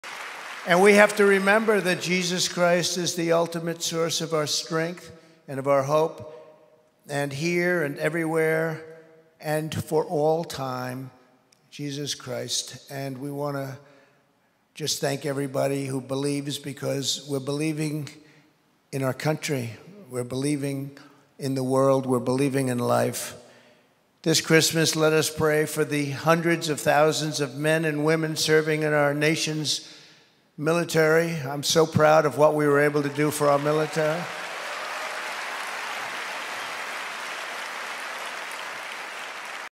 Donald Trump | Isus Hristos este sursa supremă a puterii și speranței noastre
Donald Trump | Isus Hristos este sursa supremă a puterii și speranței noastre pretutindeni și pentru totdeauna

CIF: 33686860